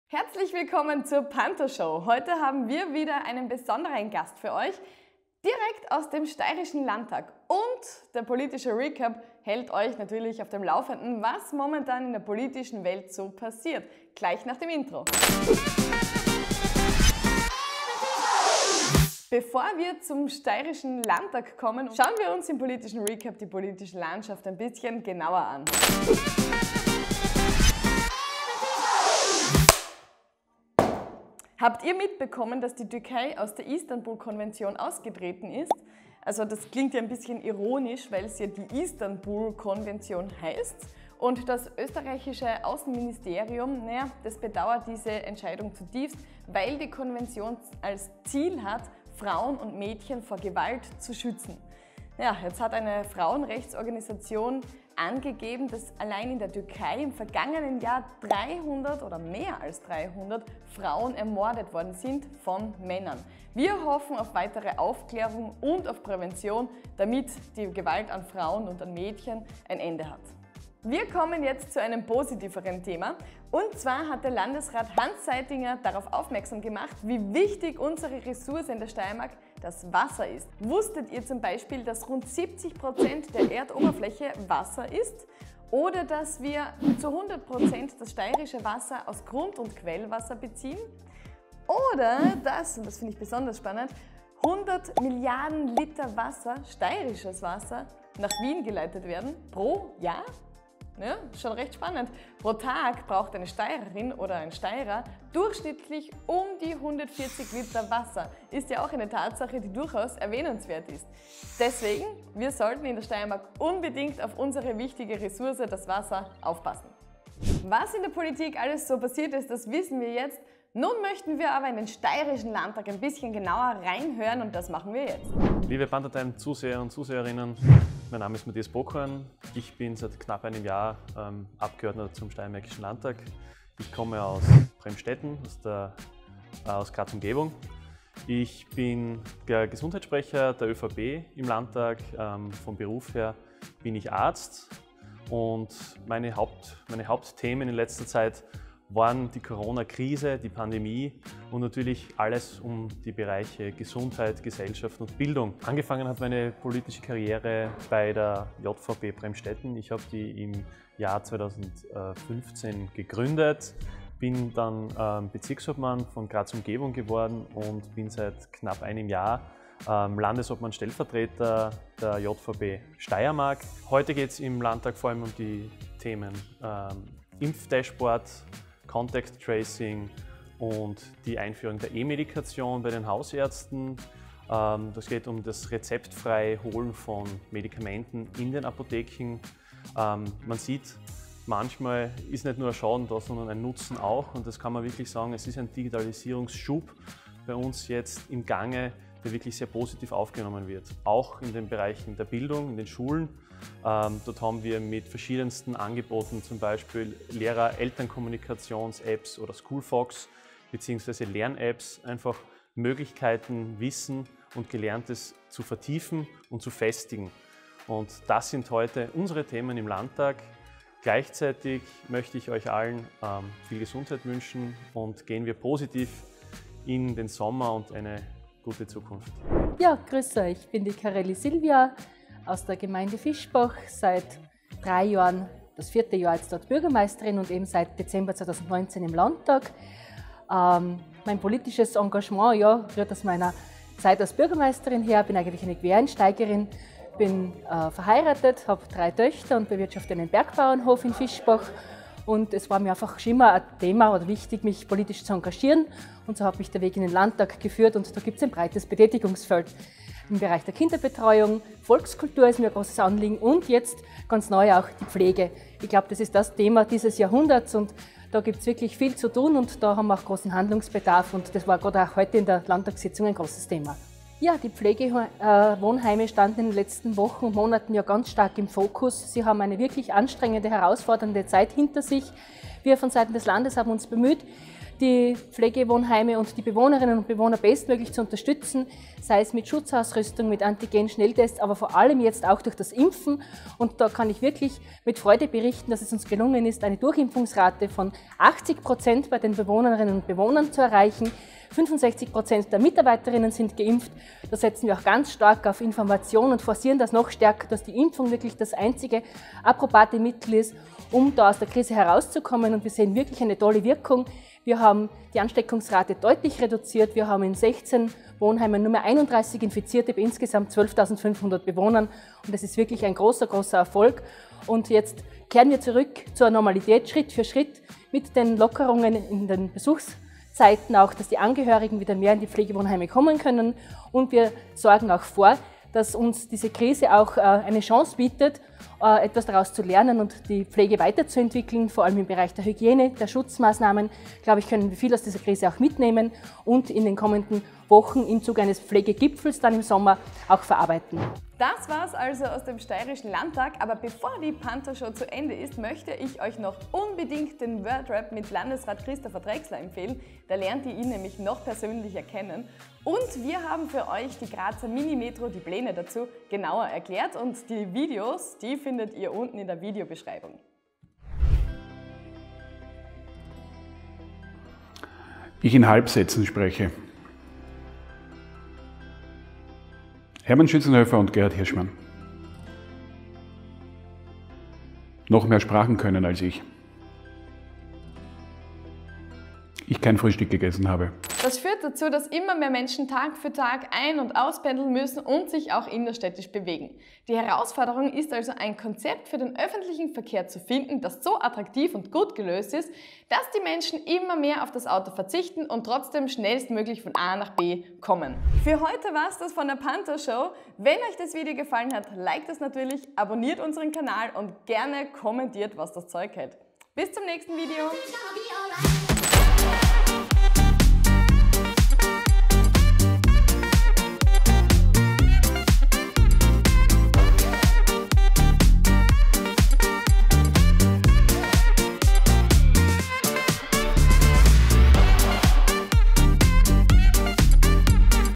Neues aus dem Landtag! - Interview mit steirischen Abgeordneten - Die Panther Show
Vergangenen Dienstag war wieder Landtagssitzung und das haben wir zum Anlass genommen zwei Abgeordnete über ihre Arbeit, ihre Themen und ihren Werdegang zu interviewen. Das Ergebnis seht ihr in unserer heutigen Panther Show!

Ein herzliches DANKESCHÖN unserem Gast:
-- SILVIA KARELLY | Abgeordnete zum steirischen Landtag

-- DR. MATTHIAS POKORN | Abgeordneter zum steirischen Landtag

Inhalte:
00:01 - Anmoderation
00:27 - POLITISCHER RECAP
02:00 - Abgeordnete aus dem steirischen Landtag stellen sich vor
06:11 - Teaser - WORDRAP mit Christopher Drexler
06:32 - Teaser - WARUM eine METRO?! - Interview mit BGM Siegfried Nagl
06:48 - Abmoderation